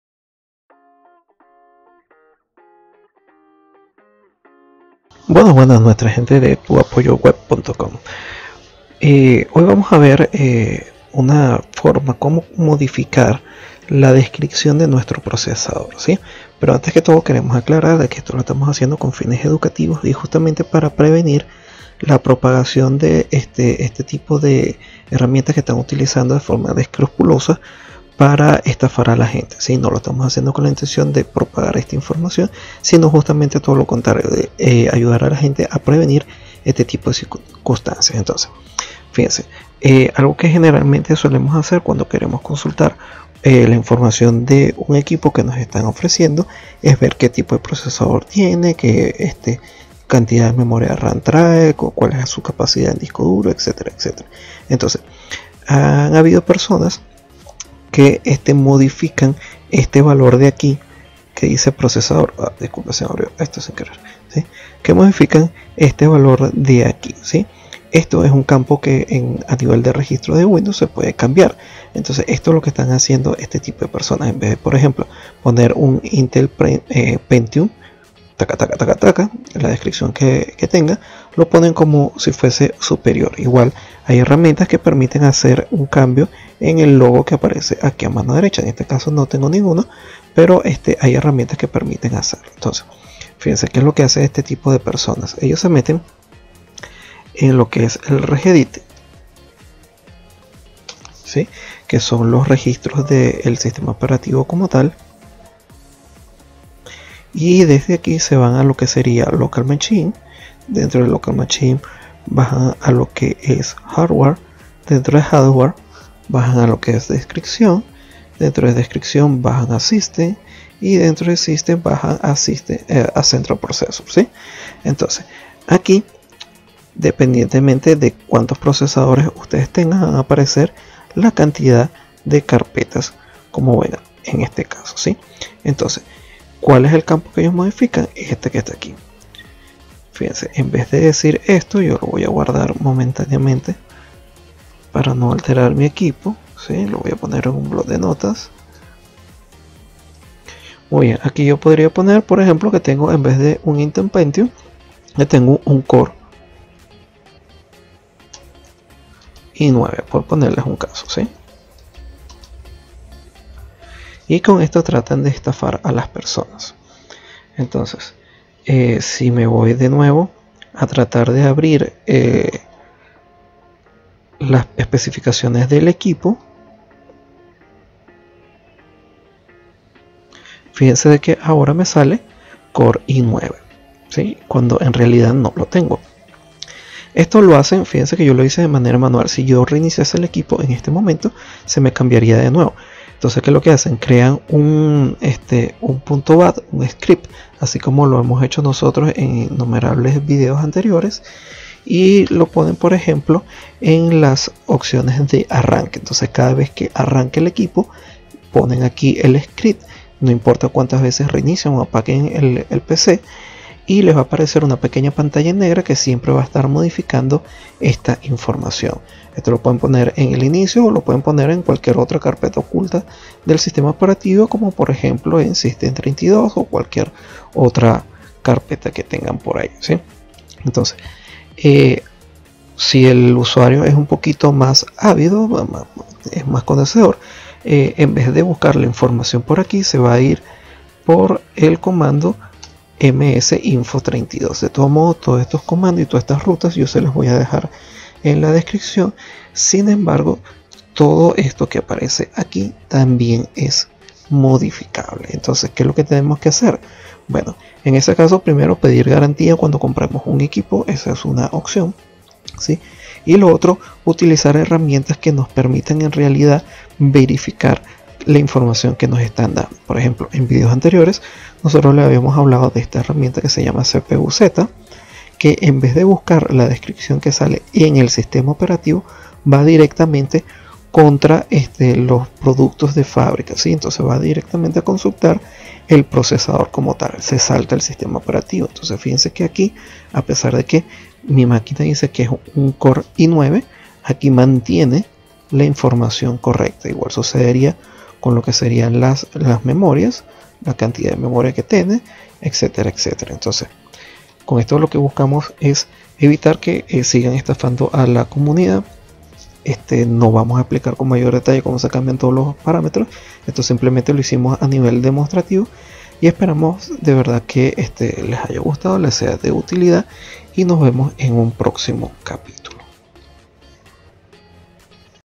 🤬Como Cambiar/ Editar la Descripción del Procesador.👺 No Hacer Esto.💀
Mediante una pequeña modificación desde el registro de Windows mostramos como se puede cambiar la descripción del CPU o procesador que sale dentro de las especificaciones técnicas de nuestro computador, para eso invitamos a las personas al inspeccionar un equipo que desean comprar evaluarlo con herramientas como CPU-Z para saber exactamente cual es el perfomance del equipo.

Comandos como MsInfo32 puede ser alterados también con este tipo de técnicas.

Hkey_Local_Machine. Hardware. Description. CentralProcessor.